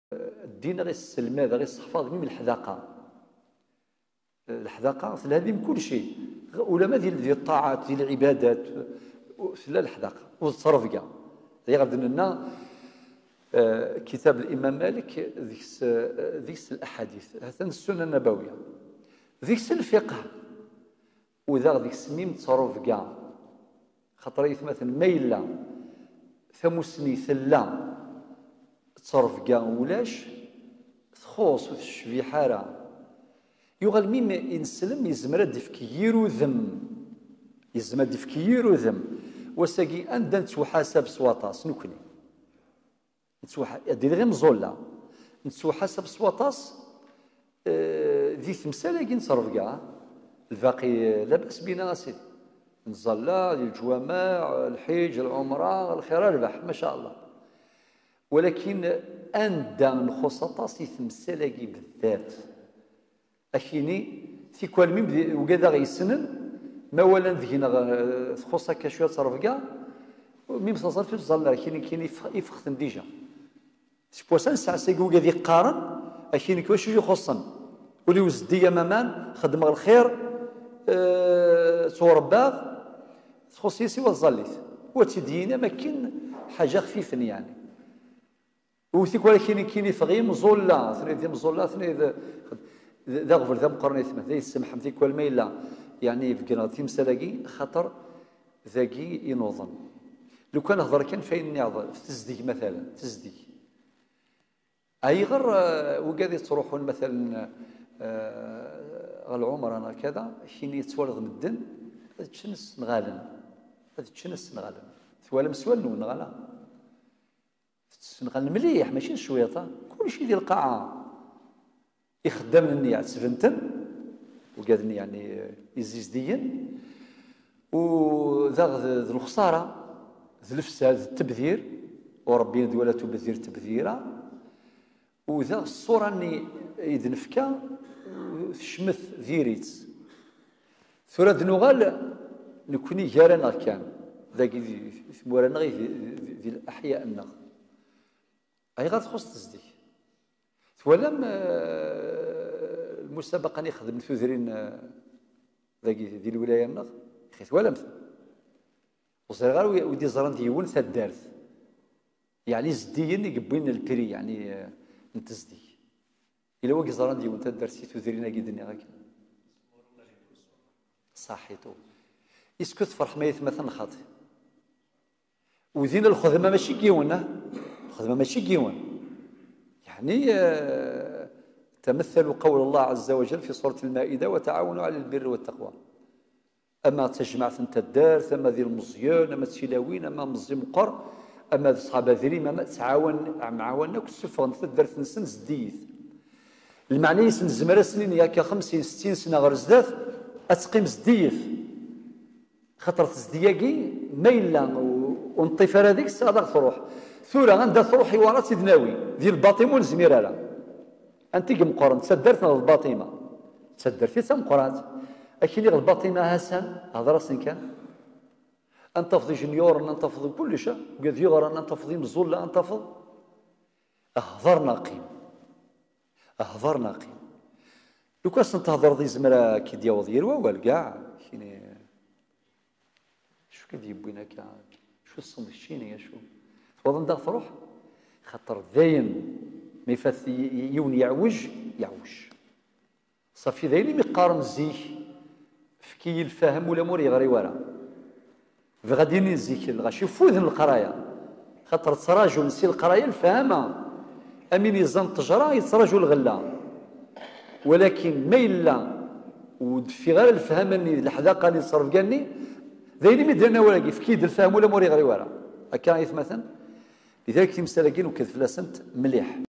Said BOUIZERI مقطع من بيان موطأ الإمام مالك 43 - الأخلاق و التربية
د.سعيد بويزري - مقطع من الموطأ 43 - الأخلاق و التربية 2019/01/04
المسجد العتيق لالة دمامية، تيزي وزو/ الجزائر